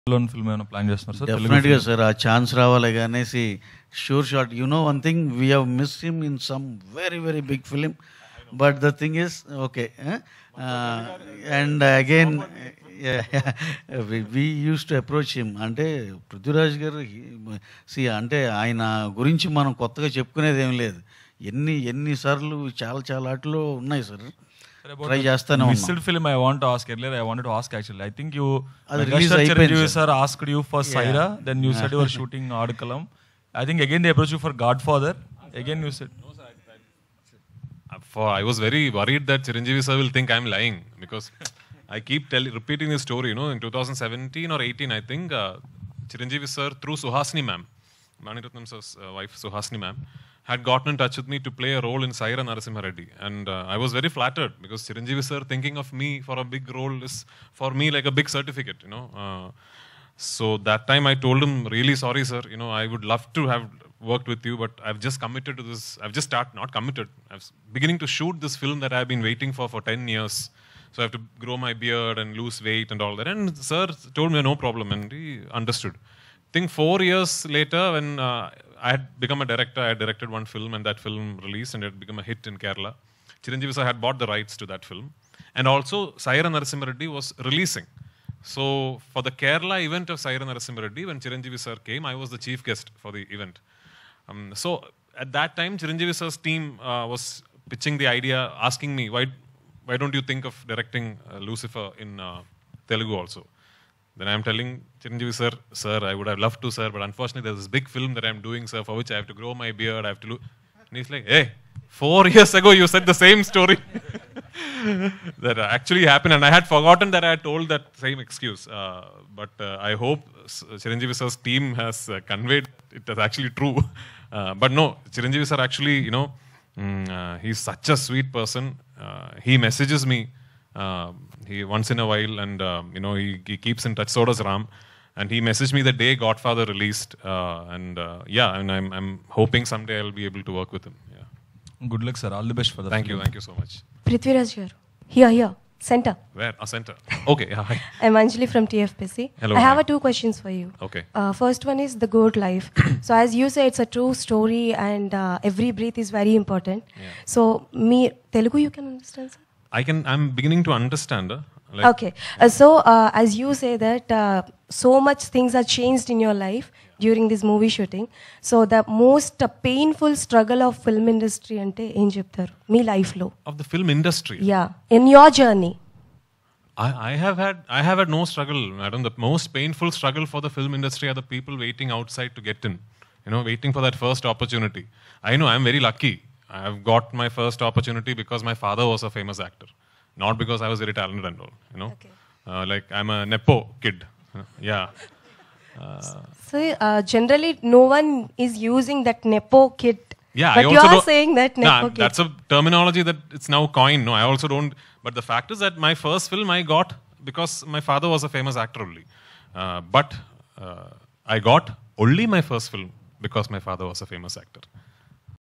Prithviraj Sukumaran Great Words About Chiranjeevi | The Goat Life Movie Pre Release Press Meet
Prithviraj Sukumaran Great Words About Chiranjeevi